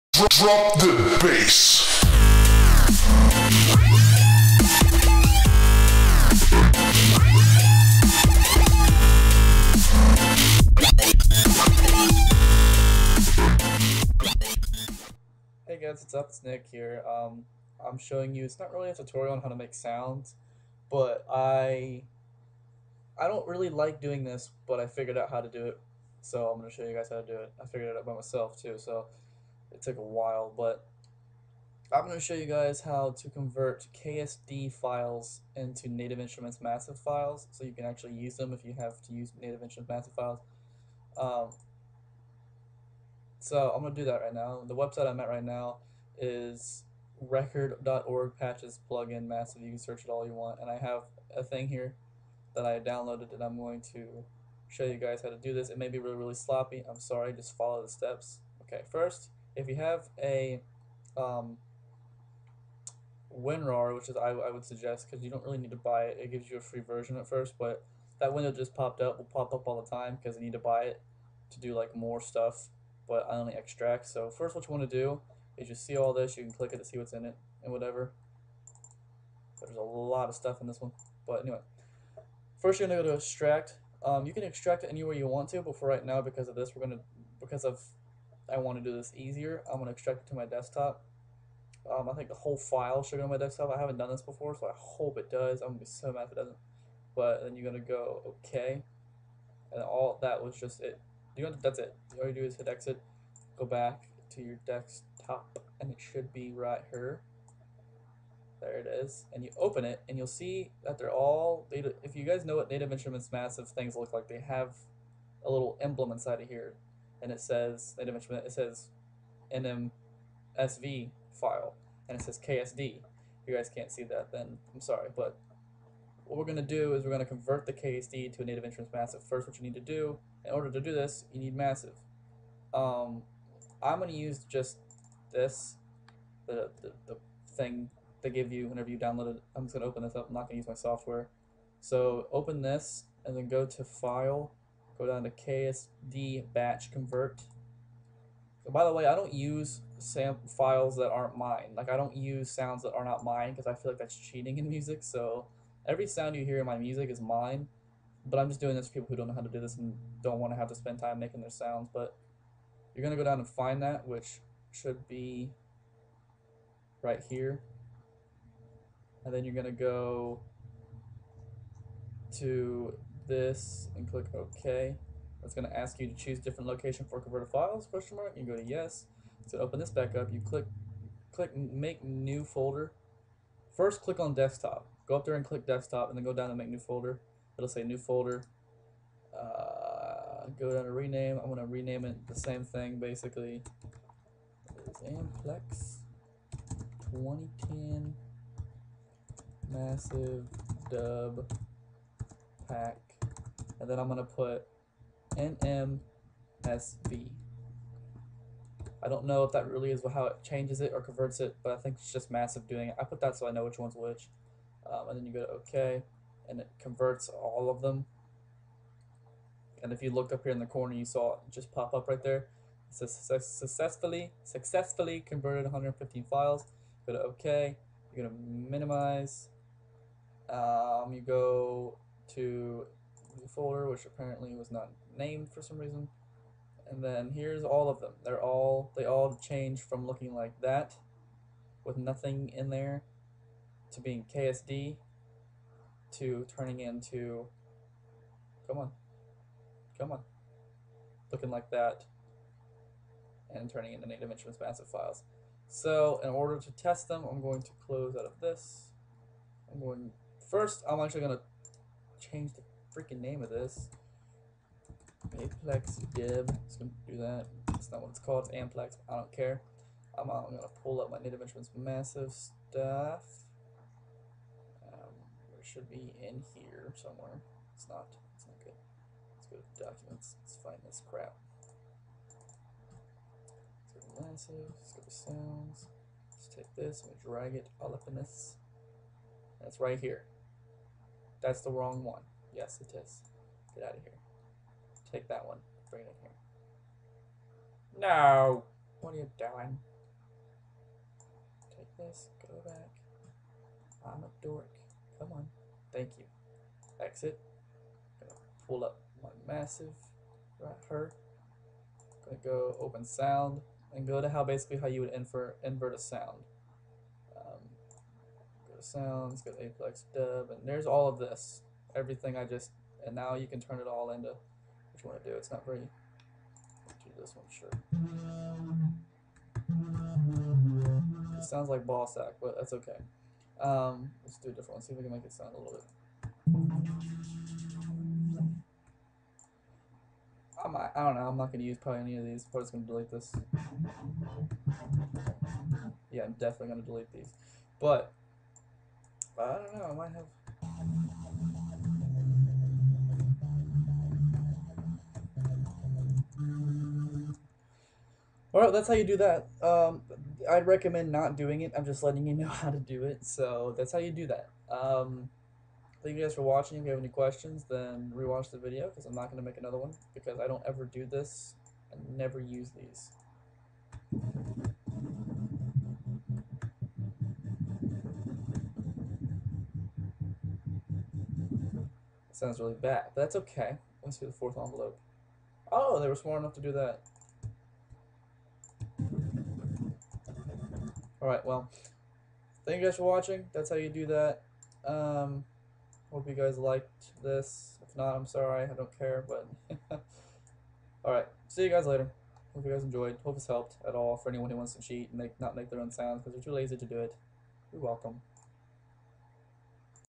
How to Convert KSD files into NMSV Massive files
I show you guys how to convert your KSD files in Massive files.